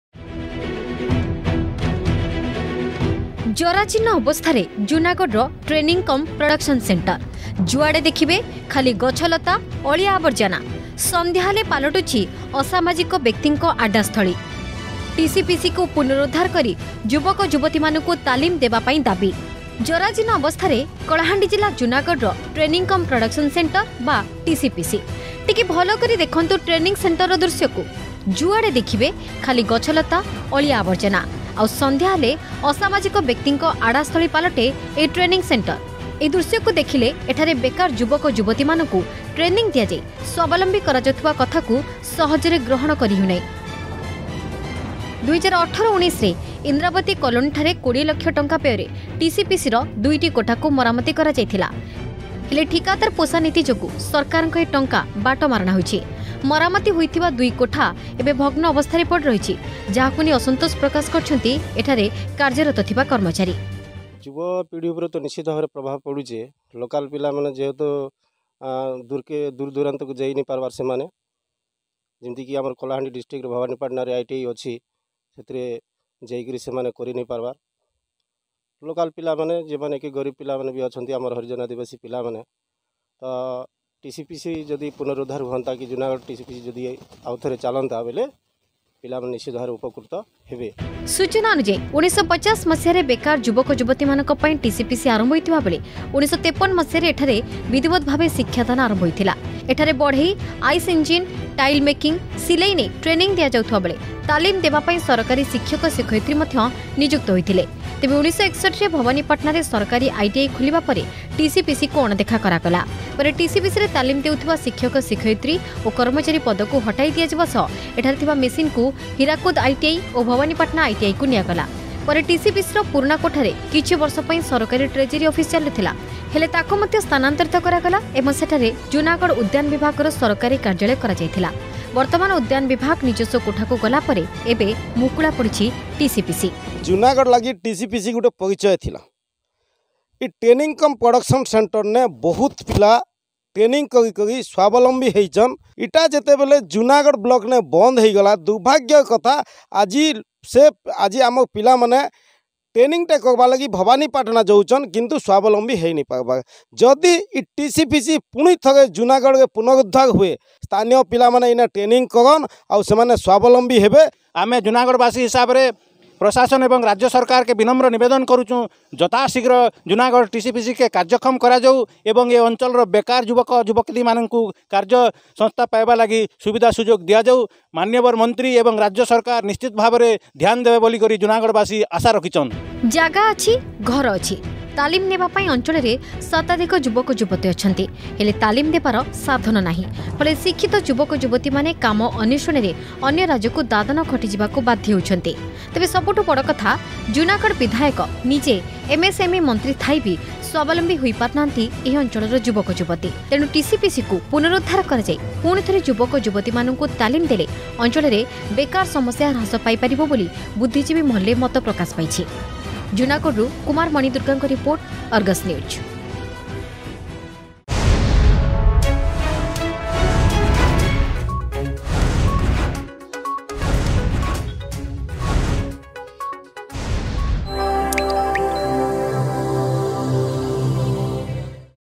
ଜରାଜୀର୍ଣ୍ଣ ଅବସ୍ଥାରେ ଜୁନାଗଡ଼ର ଟ୍ରେନିଂ କମ୍ ପ୍ରଡ଼କସନ୍ ସେଣ୍ଟର...
ଜରାଜୀର୍ଣ୍ଣ ଅବସ୍ଥାରେ କଳାହାଣ୍ଡି ଜିଲ୍ଲା ଜୁନାଗଡ଼ର ଟ୍ରେନିଂ କମ ପ୍ରଡ଼କସନ ସେଣ୍ଟର ବା ଟିସିପିସି । ଟିକେ ଭଲକରି ଦେଖନ୍ତୁ ଟ୍ରେନିଂ ସେଣ୍ଟରର ଦୃଶ୍ୟକୁ  । ଯୁଆଡେ ଦେଖିବେ ଖାଲି ଗଛଲତା, ଅଳିଆ ଆବର୍ଜନା...